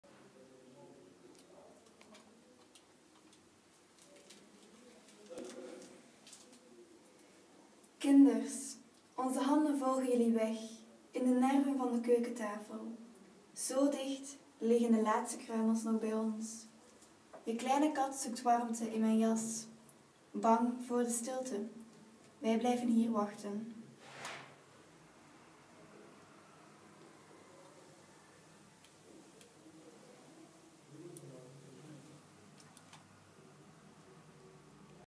Ma Willoqué aan haar zonen Frans en Hubert - Anne Cockaerts
kinders,

onze handen volgen jullie
weg in de nerven
van de keukentafel
zo dicht
liggen de laatste kruimels
nog bij ons
je kleine kat zoekt warmte
in mijn jas
bang voor de stilte

wij blijven hier
wachten

(Voordracht: Ine De Man - Regie: Patrick Bernauw & Mirjam Van Lith)